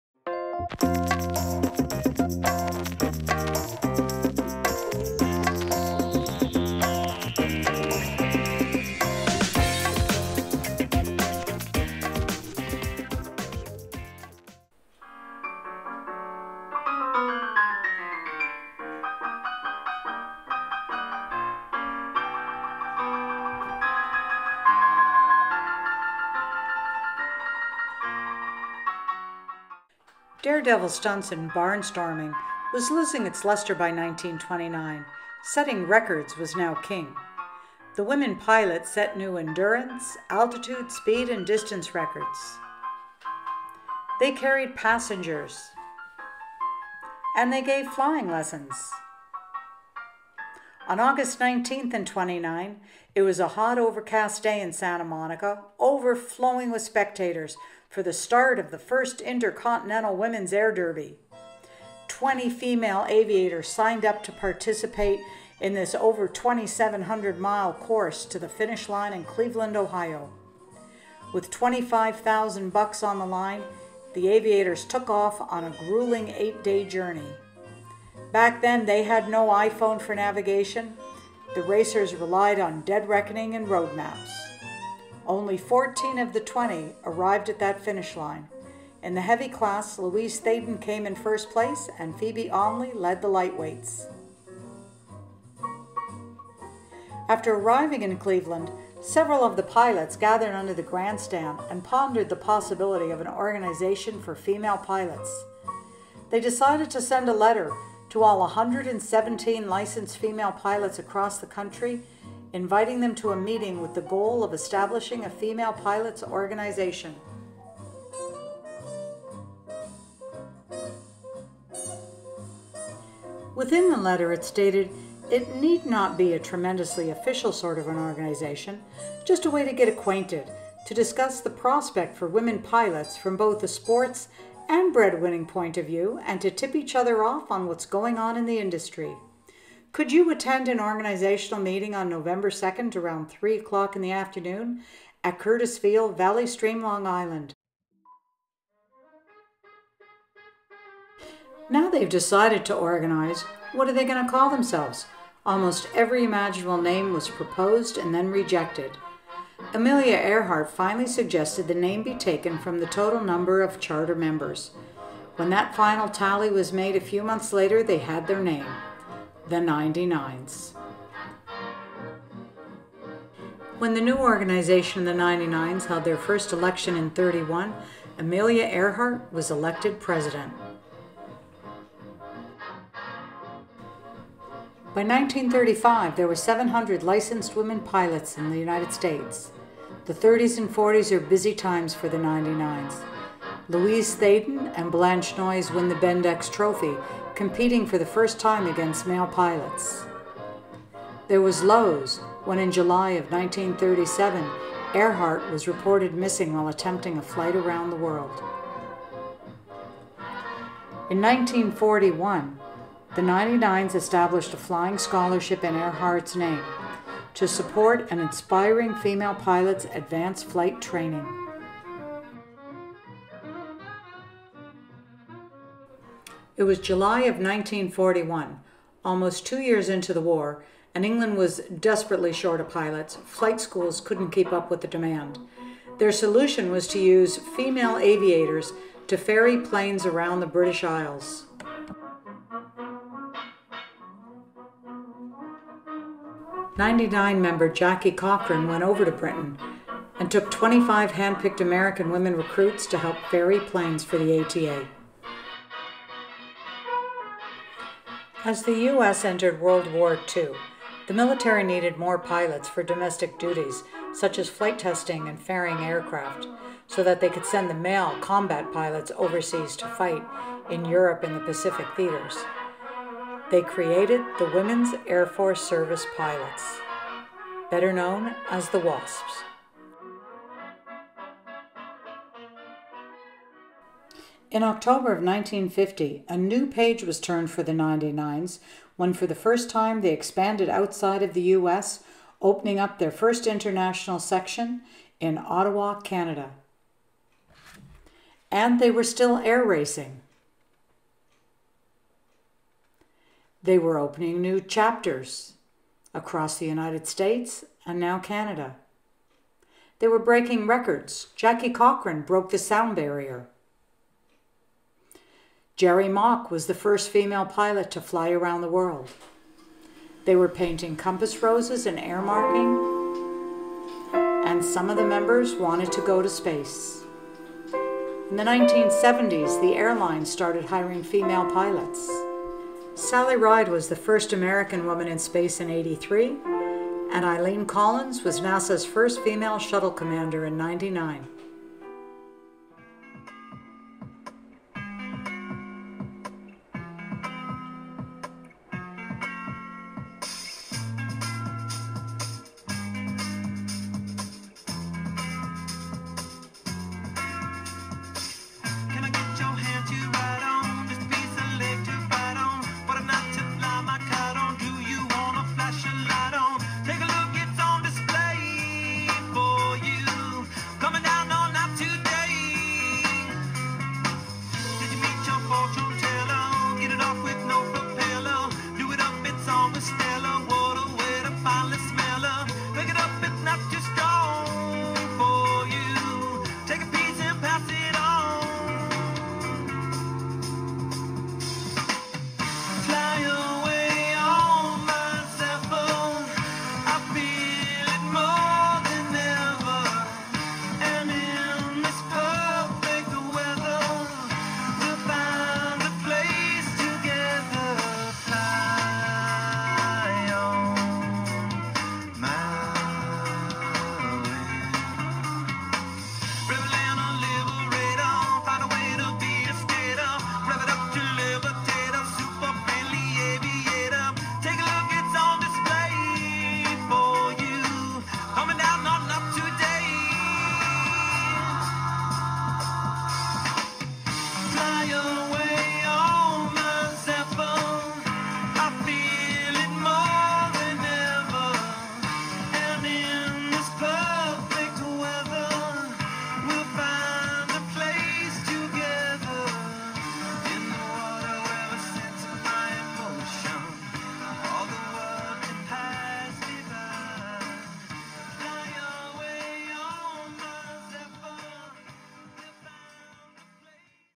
The Ninety-Nines (99s) 90th Anniversary, International Organization of Women Pilots
Celebrating 90 years of the Ninety-Nines Inc, International Organization of Women Pilots. A quick history from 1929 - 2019.